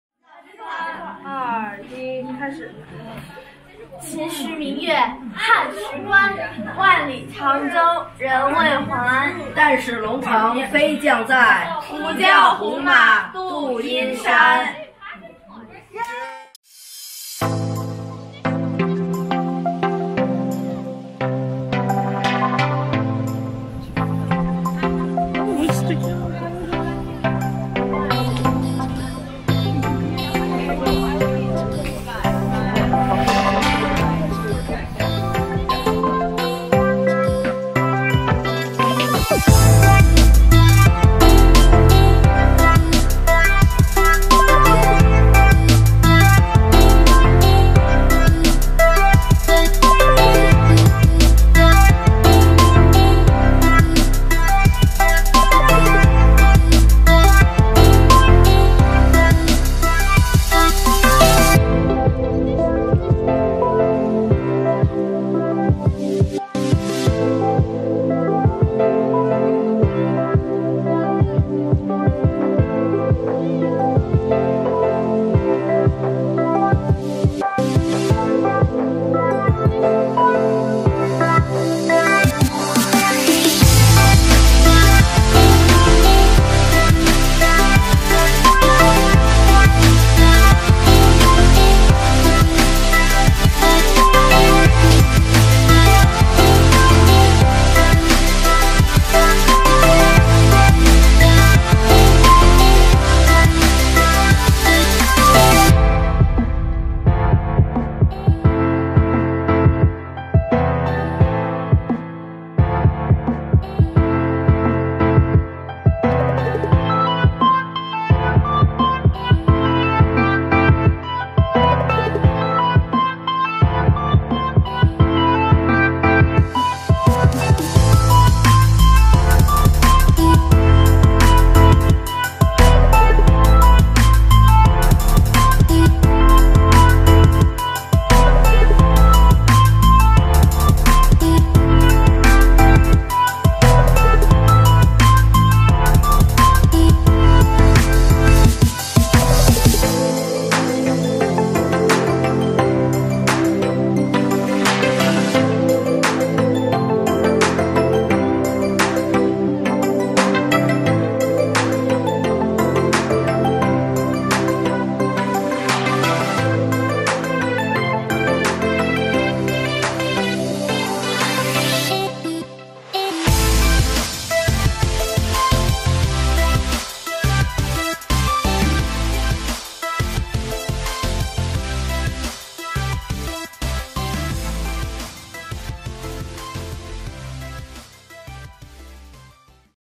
China, November 2019: GelKay Travels The Great Wall Of China, Beijing 🇨🇳
Travel to The Great Wall in Beijing, China. November 2019.